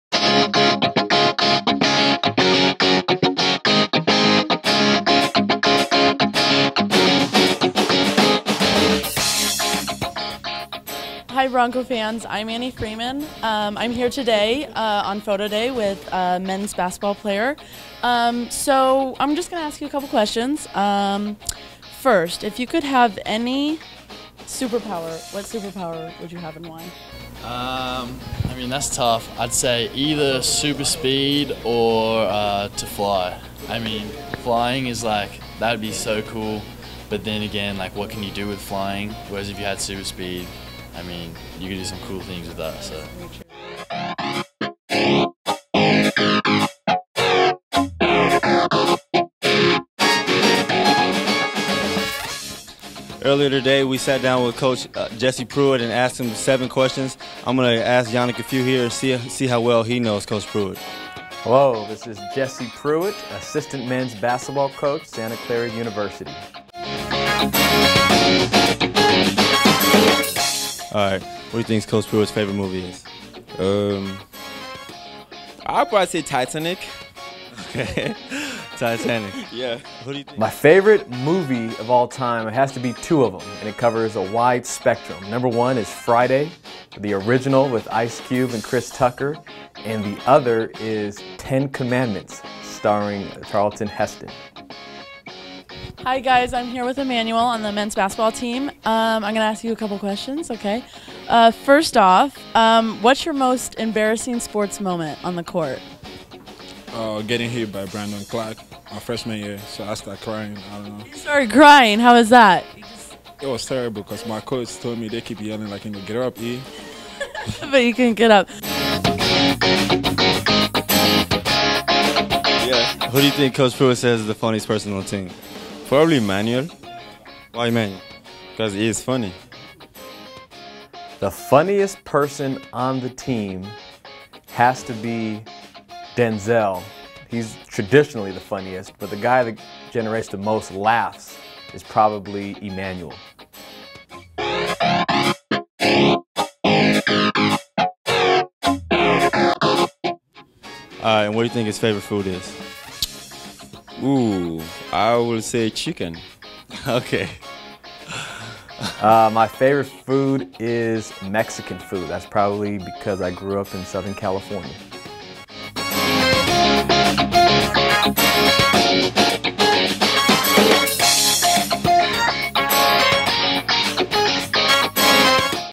EYEBRONCO: Santa Clara Men's Basketball Photo Day
Check out Santa Clara men's basketball at their photo day! Yannick Atanga sees how well he knows assistant coach Jesse Pruitt, Emmanuel Ndumanya shares his most embarrassing moment on the court, and Kai Healy chooses a super power!